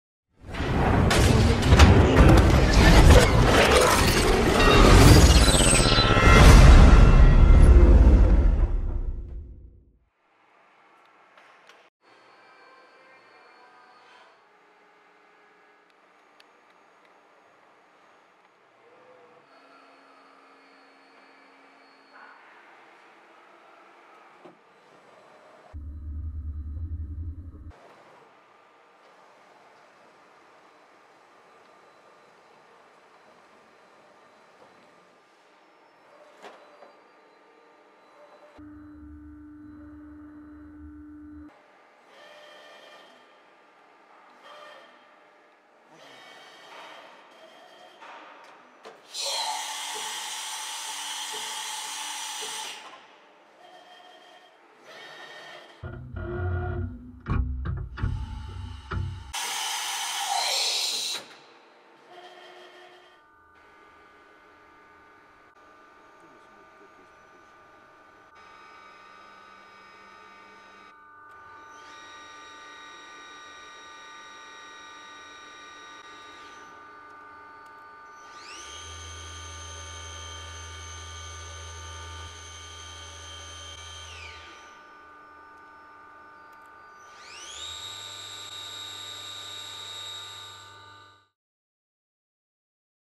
VMC Schaffner VC12 / Fanuc
Vertical machining center Schaffner VC12
YOM: 1990
Control: Fanuc 0 Series
X: 1200 mm
Y: 450 mm
Z: 450 mm
Fixed clamping table: 2000 x 450 mm
Tool holders: SK 40
Spindle: 6000 rpm
ATC 24 stations